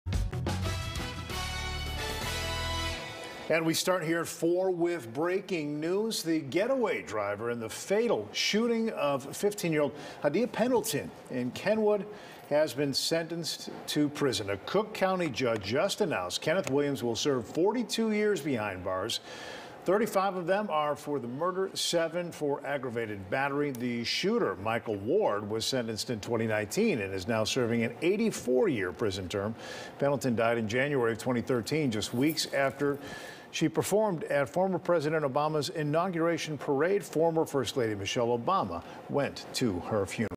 Getaway driver in deadly shooting of 15-year-old Hadiya Pendleton sentenced to 42 years
The getaway driver in the deadly shooting of 15-year-old Hadiya Pendleton was sentenced to 42 years in prison on Tuesday.